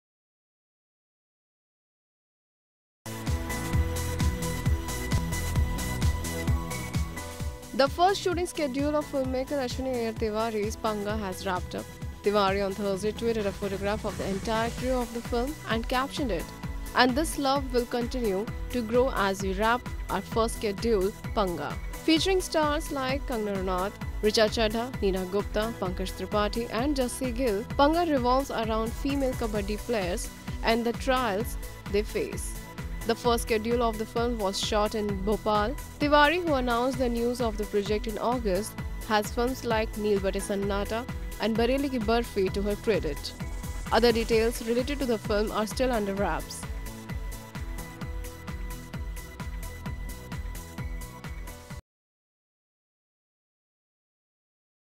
Kangana, Richa’s “PANGA”, First SCHEDULE Wrapped up
The first shooting schedule of filmmaker Ashwiny Iyer Tiwari's "Panga" has wrapped up. Featuring stars like Kangana Ranaut, Richa Chadha, Neena Gupta, Pankaj Tripathi and Jassie Gill, "Panga" revolves around female kabaddi players and the trials they face.